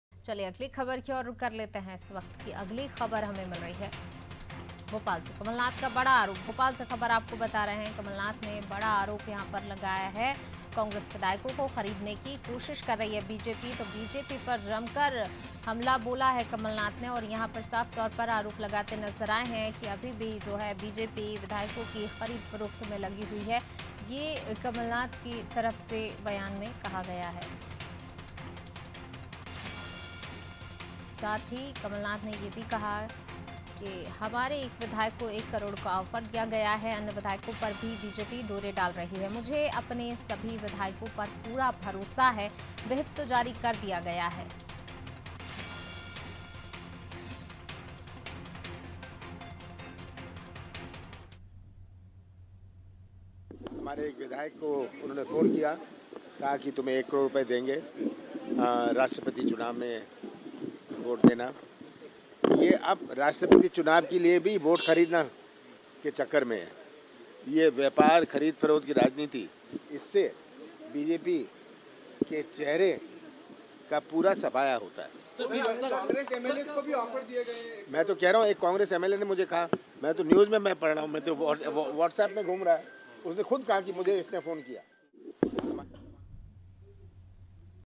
कांग्रेस विधायकों को खरीदने की कर रही कोशिश बीजेपी : कमलनाथ
भोपाल : कमलनाथ का बड़ा आरोप
‘कांग्रेस विधायकों को खरीदने की कर रही कोशिश बीजेपी’
‘अन्य विधायकों पर भी डोरे डाल रही बीजेपी’
‘मुझे अपने सभी विधायकों पर पूरा भरोसा’
व्हिप तो जारी कर दिया गया है- कार्रवाई